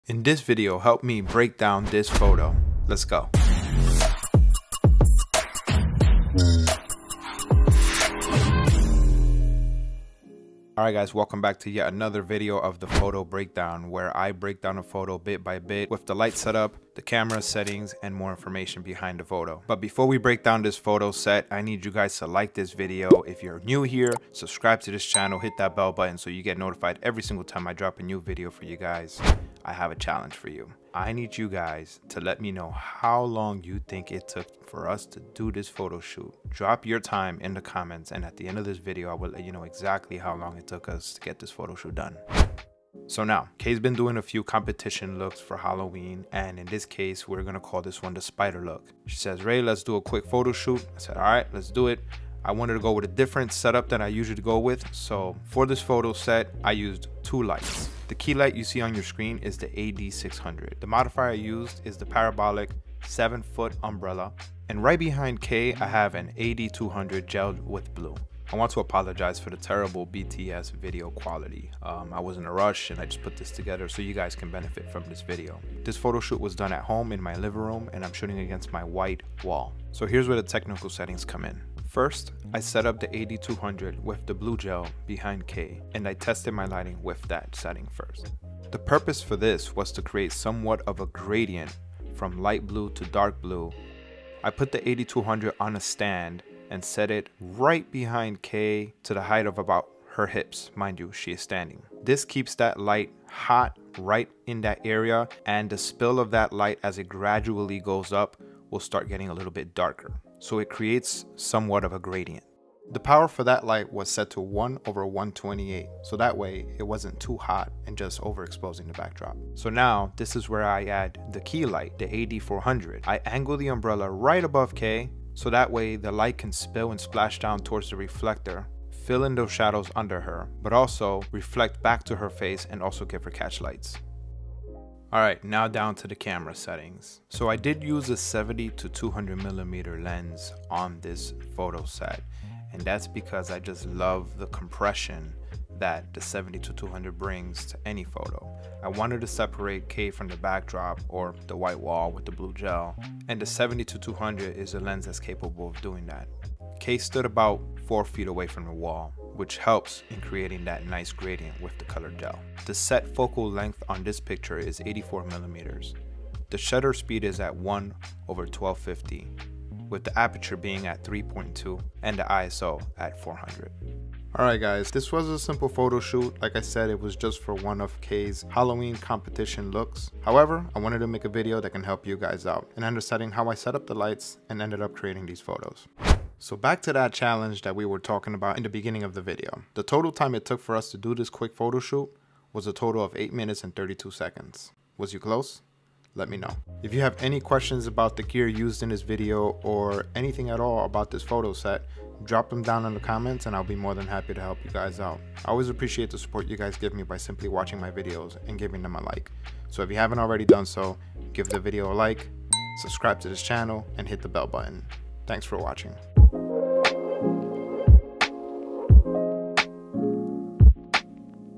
Creating Gradients with two lights - Photo Breakdown
Another Photo Breakdown video. This time it's the Halloween Edition where we work with Kay on one of her recent competition looks which we are titling "The Spider Look"

Join me in breaking down this photo. Let's talk about how many lights I used, the camera settings and how I created a gradient using said lights.

💄SFX Makeup Artist: Kay Alvarez

📷 Lighting, Camera & Gear Info:
Sony a7rv
70-200mm 2.8GM

*You will need a transmitter to use these units, drop your brand in the comments and I can link you to the transmitter you will need

Impact 7' Parabolic Umbrella
MagMod Creative Gels
Angler Curved Reflector

I got way more coming. If you have any questions.

📣 Social Media - Give me a follow!
Portrait Portfolio
Wedding Portfolio